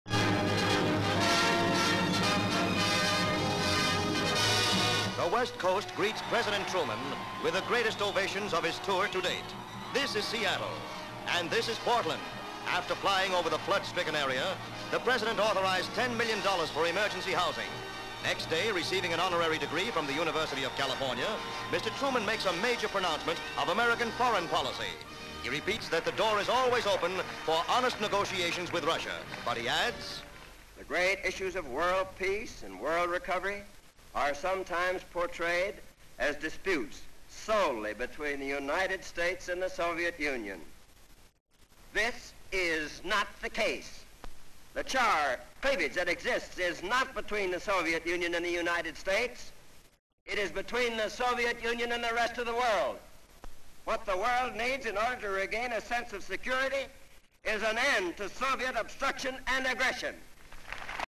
Truman On Foreign Policy AKA Truman Speaks On Foreign Policy (1948)
Seattle (Washington), Portland (Oregon) and Berkeley (California), United States of America

TRUMAN ON FOREIGN POLICY

During his tour of the country, President Harry Truman receives an honorary degree from the University of California and addresses students to talk about foreign policy.

L.S. Elevated shot of Thames procession passing through Seattle streets. L.S. Elevated shot of procession through Portland streets. L.S. Truman's procession passing through the streets of Berkeley California. M.S. Ditto.  M.S. Truman in professor's robes and mortar board, on open air rostrum at California University :

Speech: "The great issues of world peace and world recovery are sometimes portrayed as distinct solely between the United States and the Soviet Union.  This is not the case.  The charge of prejudice which exists is not between the Soviet Union and the United States.  It is between the Soviet Union and the rest oft he world. What the word needs to regain a sense of security is an end to Soviet obstruction and aggression".

B/W. Lav.
 FILM ID:2449.1